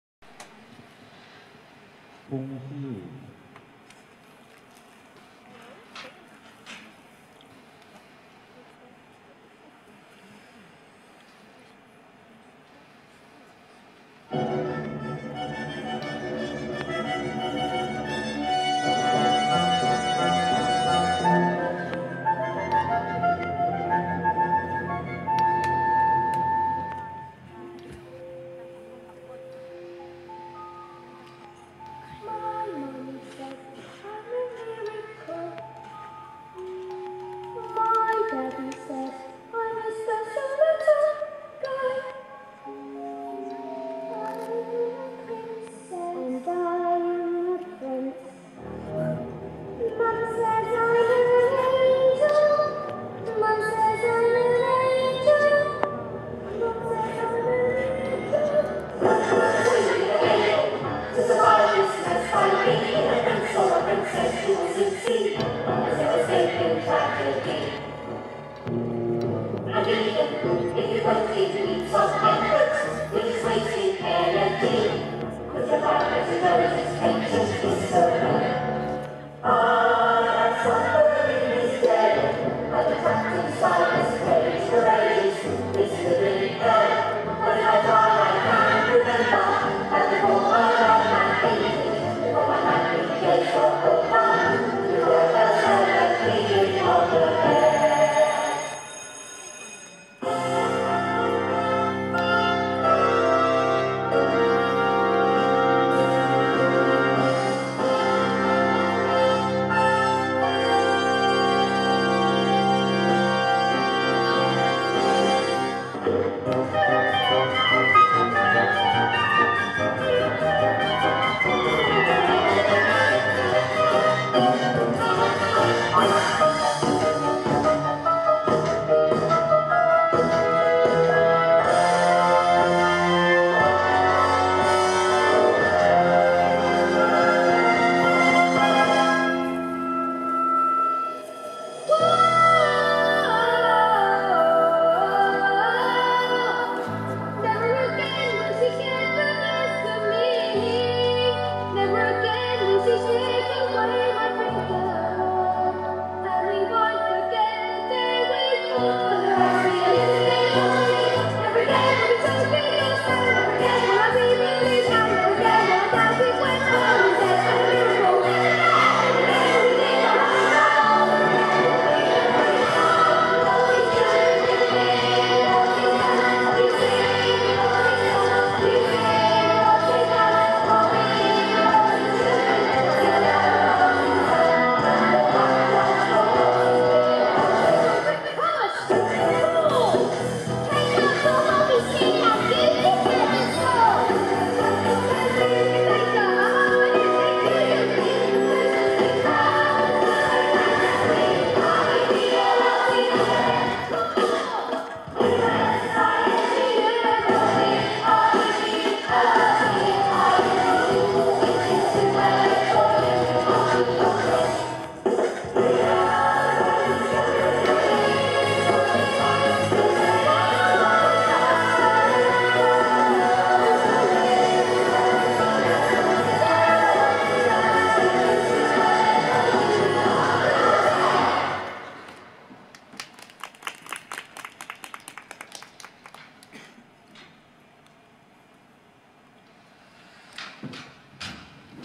송시우 Si Woo SONG (Si-Woo) | 6급 FS 심사 5 | 2019-11-30 4차 5-8급 승급심사 | 벌말초 6
Figure Skating Level Test 2019. 4th. | (30. Nov.) 2019. Taereung Indoor Ice Rink
피겨스케이팅 4차 5-8급 승급심사 | 태릉실내빙상장